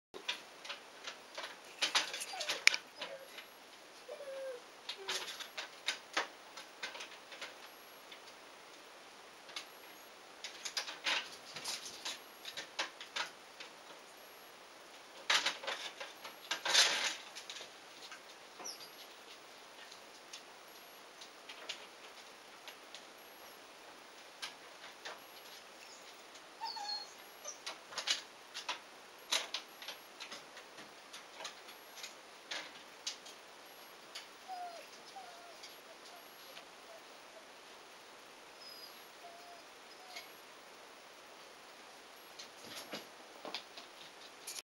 ラブラドールPUPPYの脱走
下にベッドがあるところまでちゃんと歩きます。
猫か！？